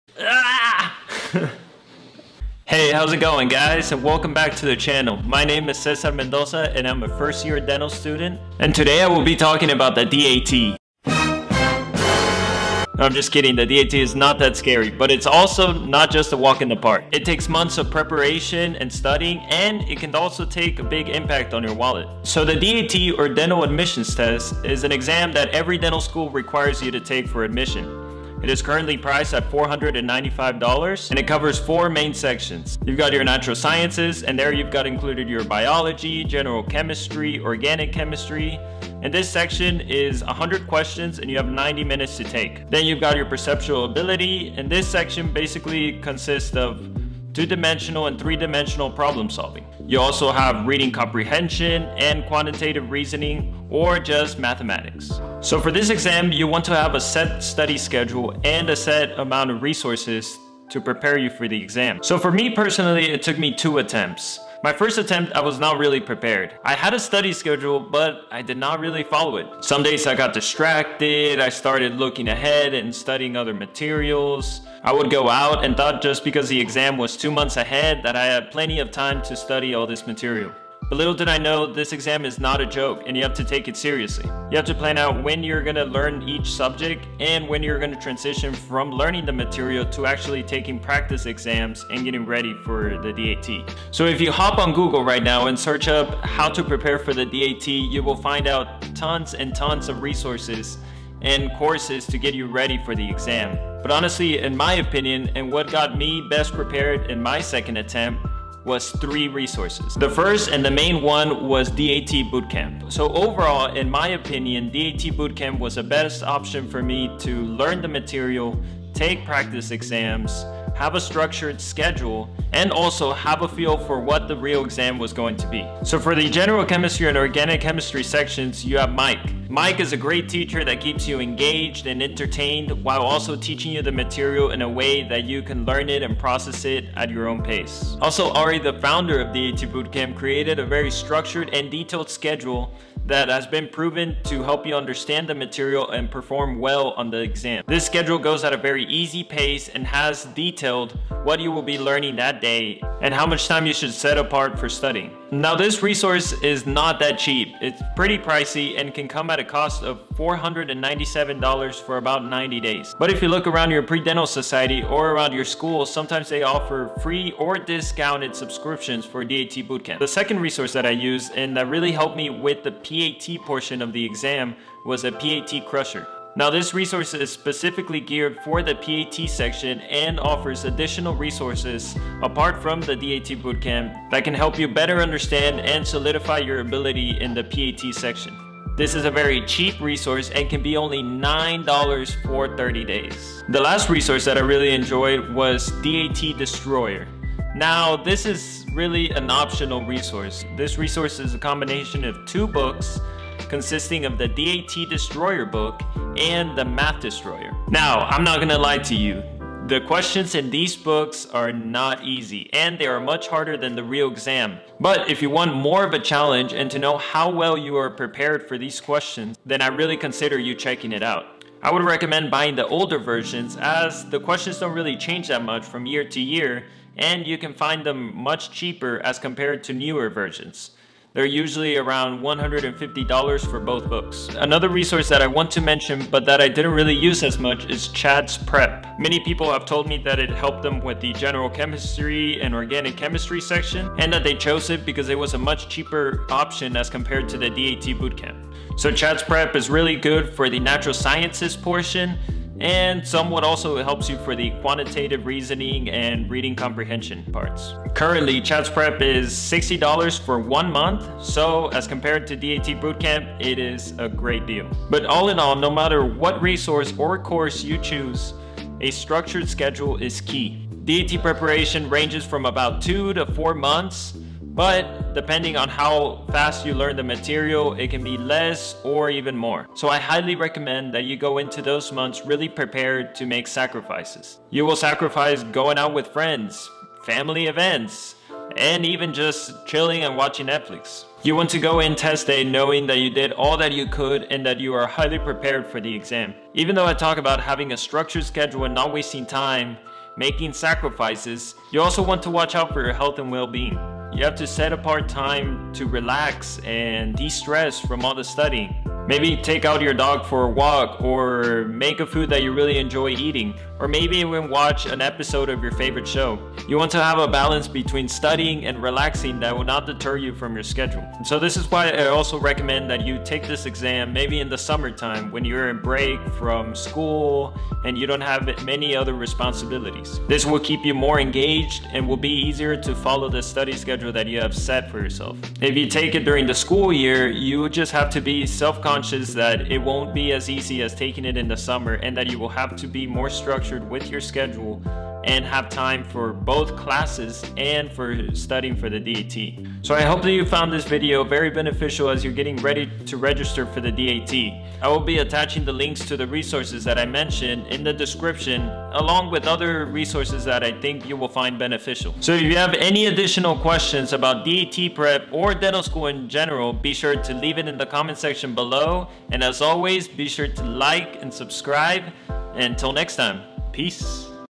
HOW TO STUDY for the DAT (Ultimate Guide)
Cesar breaks down how to exactly study for the DAT (Dental Admission Test), the TOP RESOURCES to use, and some tips to help you ACE IT!

The DAT is without a doubt an important and often daunting exam. However if you put in the necessary work and apply the tips discussed in this video you will go into exam day much more confident and much more likely to achieve your target score. You have what it takes!

Links to content discussed:
📚 DAT Bootcamp: Top resource in my opinion (reasons discussed in video):

Stay FLOSSY my peeps...